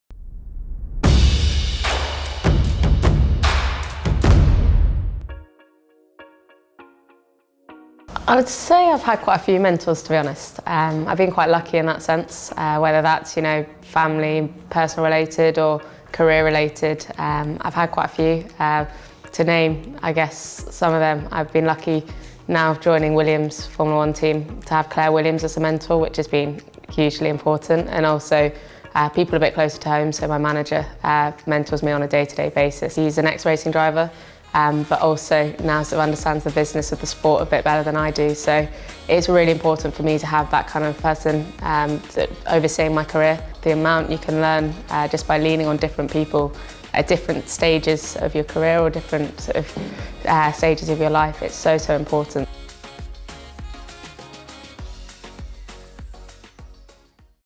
Jamie Chadwick shares her thoughts on mentoring
British racing driver and W Series Champion, Jamie Chadwick, talks to us about mentoring, how it’s benefited her career and the impact it’s had on her success.